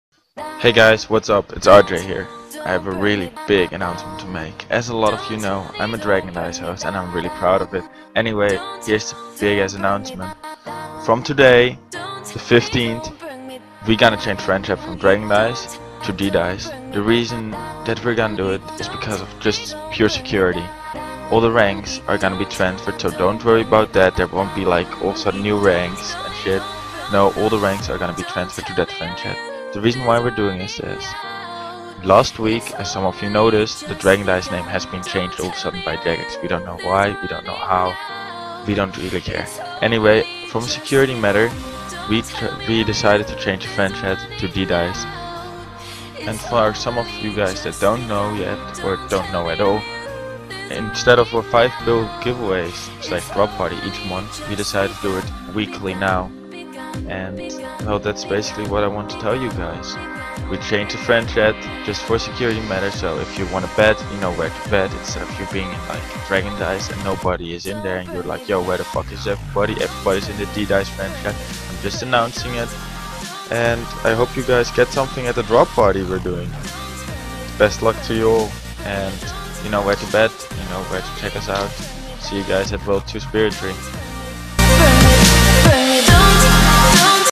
Dragon dyce Changing to "DDyce" New Friends Chat
We are moving to a new Friends chat, the new name is  "DDyce"